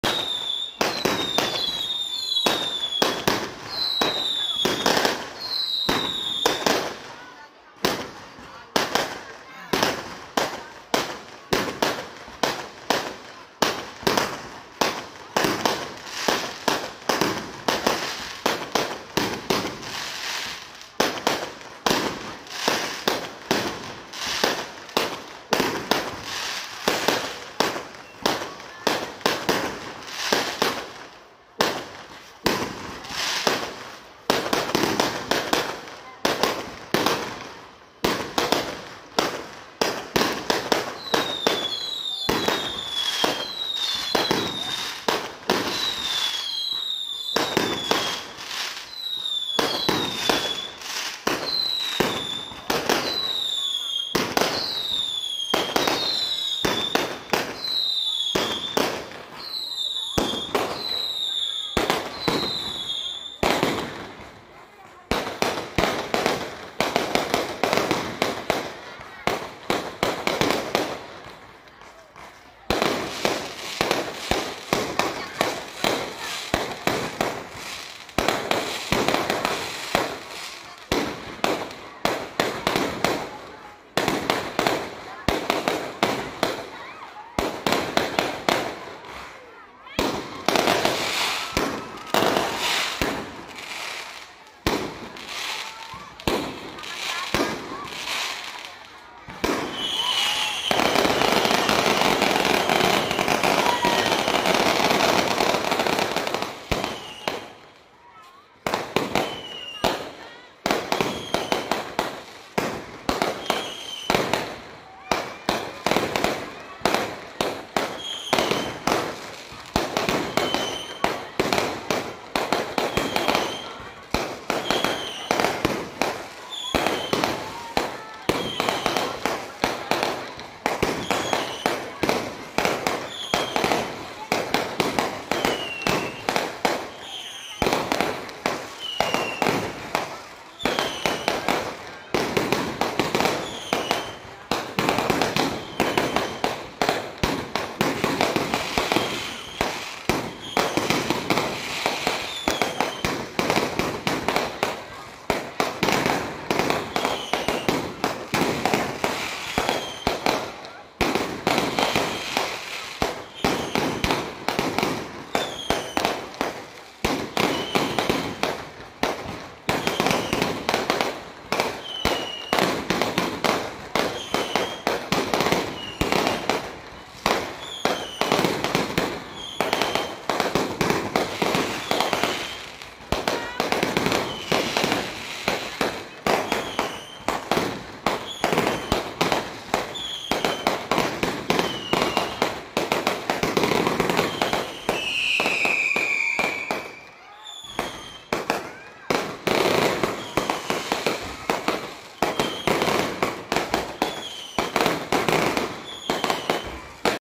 MERIAH, PENUTUPAN MTQ MUARA JAWA ( Dondang ) 2022, KEMBANG API 10 MENIT 🤩
Penutupan MTQ di kelurahan dondang ( Muara Jawa ) di tutup dengan kemeriahan dengan kembang api ( Berdurasi 10 menit ) yang ditandainya selesai sudah pagelaran MTQ kali ini 🤩 Alhamdulillah.